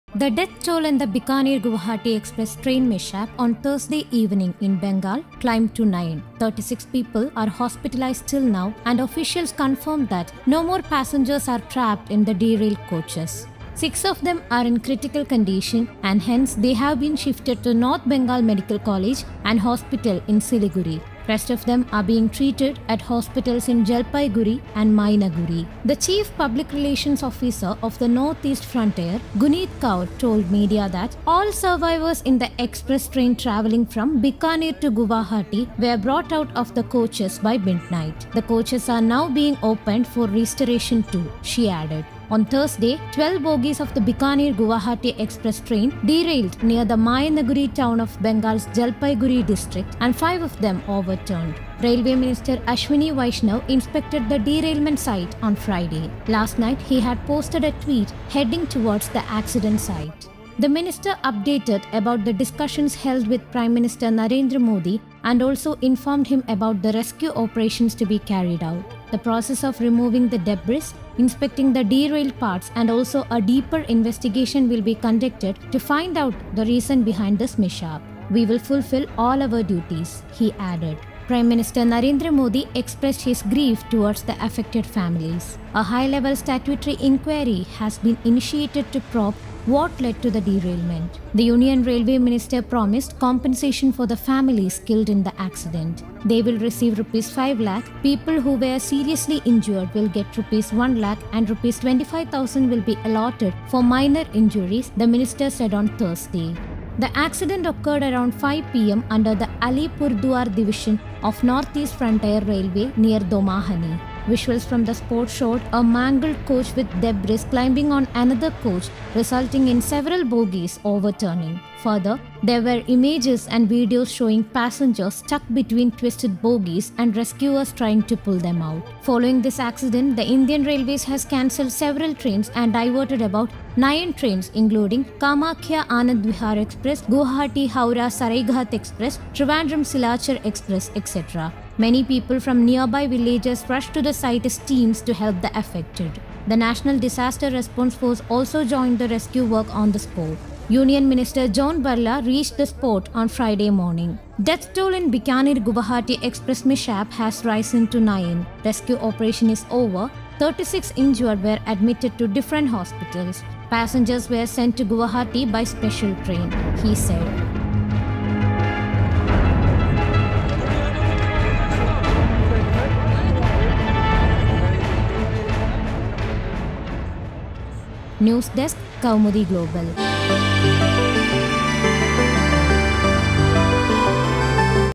Bikaner-Guwahati Express train mishap: Death toll climbs to nine; 36 Injured
The death toll in the Bikaner-Guwahati Express train mishap on Thursday evening in Bengal climbed to nine. 36 people are hospitalised till now and officials confirmed that no more passengers are trapped in the derailed coaches. Six of them are in critical condition and hence, they have been shifted to North Bengal Medical College and Hospital in Siliguri.
 Rest of them are being treated at hospitals in Jalpaiguri and Maynaguri.

The chief Public relations Officer of the North East Frontier, Guneet Kaur told media that ‘All survivors in the Express train travelling from Bikaner to Guwahati were brought out of the coaches by midnight. The coaches are now being opened for restoration too, she added.

On Thursday, Twelve bogies of the Bikaner-Guwahati Express train derailed near the Maynaguri town of Bengal's Jalpaiguri district, and five of them overturned.
Railway Minister Ashwini Vaishnaw inspected the derailment site on Friday. Last night he had posted a tweet "Heading towards the accident site".

The minister updated about the discussions held with Prime Minister Narendra Modi and also informed him about the rescue operations to be carried out. “The process of removing the debris, inspecting the derailed parts, and also a deeper investigation will be conducted to find out the reason behind this mishap. "We will fulfil all our duties," he added.

Prime Minister Narendra Modi expressed his grief towards the affected families. A high-level statutory enquiry has been initiated to probe what led to the derailment.
The Union Railway Minister promised compensation for the families killed in the accident. They will receive Rs 5 lakh, people who were seriously injured will get Rs 1 lakh and Rs 25,000 will be allotted for minor injuries, the minister said on Thursday.

The accident occurred around 5 pm under the Alipurduar Division of Northeast Frontier Railway near Domahani. Visuals from the spot showed a mangled coach with debris climbing on another coach resulting in several bogies overturning. Further, there were images and videos showing passengers stuck between twisted boggies and rescuers trying to pull them out.
Following this accident, the Indian Railways has cancelled several trains and diverted about 9 trains including Kamakhya Anand-Vihar Express, Guwahati-Howrah Saraighat Express, Trivandrum-Silachar Express etc. Many people from nearby villages rushed to the site as teams, to help the affected. The National Disaster Response Force also joined the rescue work on the spot.

Union Minister John Barla reached the spot on Friday morning. "Death toll in Bikaner-Guwahati Express mishap has risen to 9. Rescue operation is over. 36 injured were admitted to different hospitals. Passengers were sent to Guwahati by special train." he said.